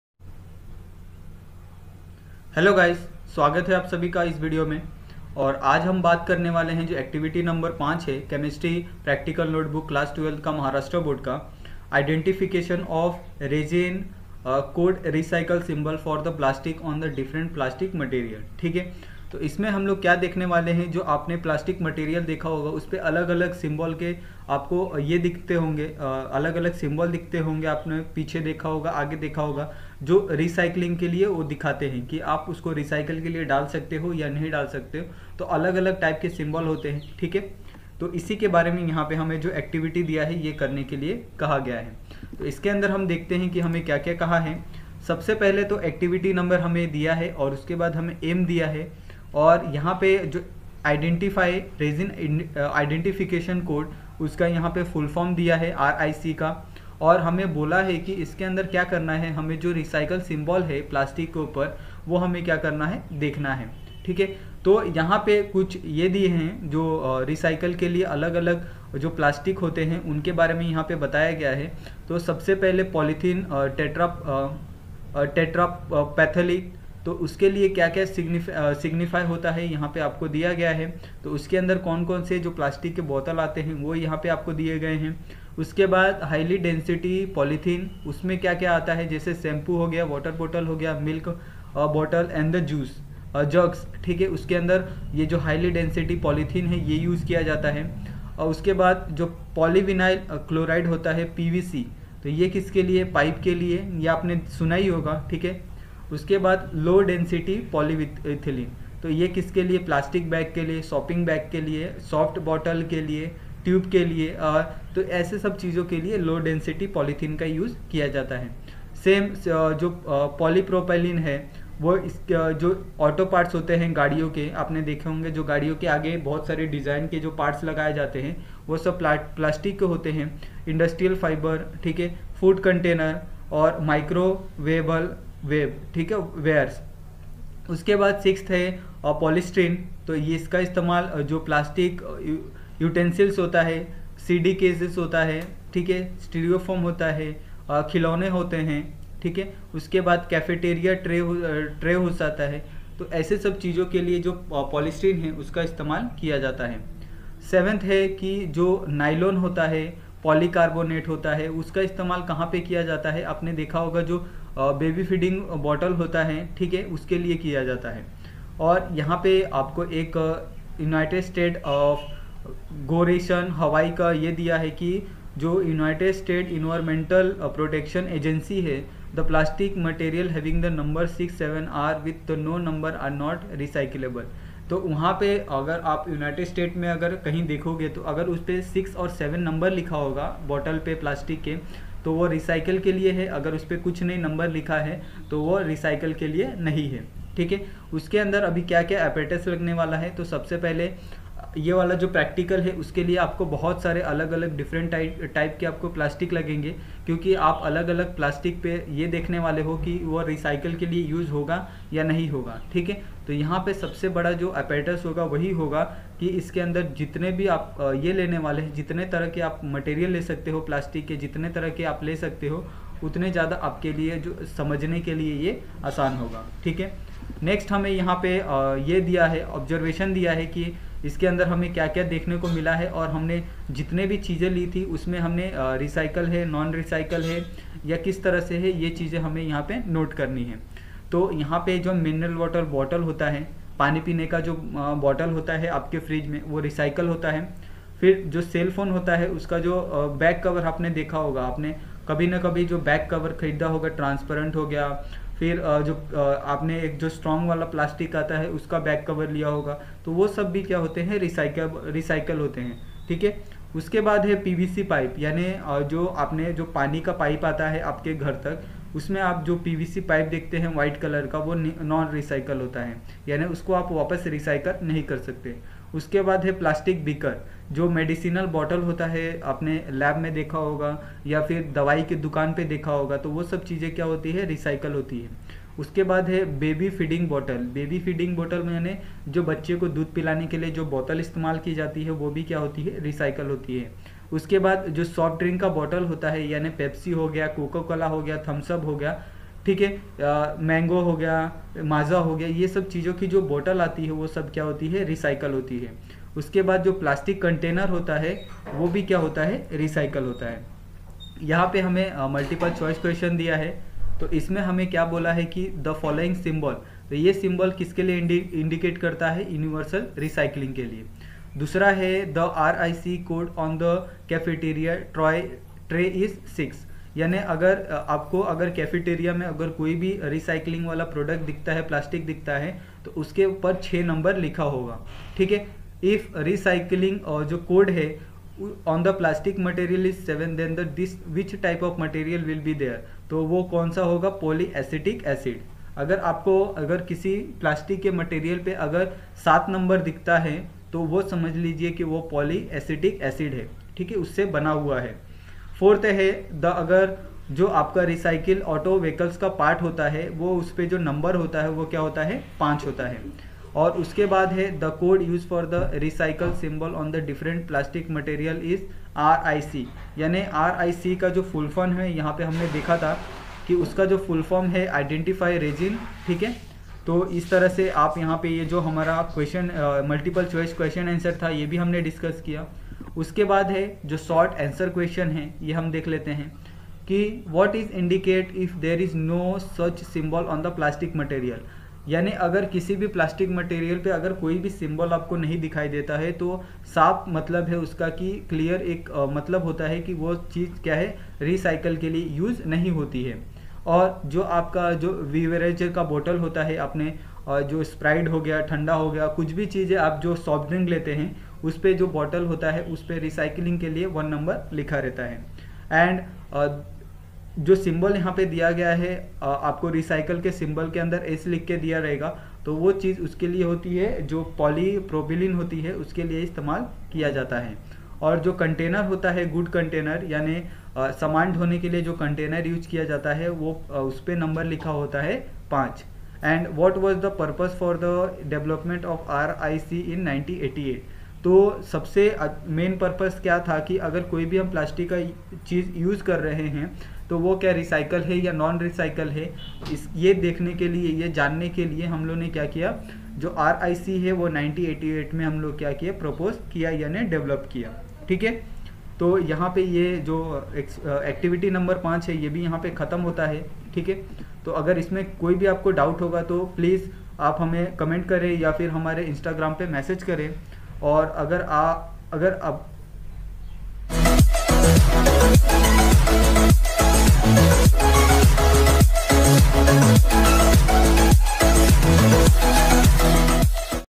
ACTIVITY NO:- 5 - 12th Chemistry / Resin identification code recycle symbol of plastic / HSC Board
ACTIVITY NO:- 05 - 12th Chemistry / Resin identification code recycle symbol of plastic / HSC Board.

MYSELF ,
NILESH PRASAD FROM BOISAR ( PALGHAR DISTRICT )
PLZ SUPPORT ME FRENDS ,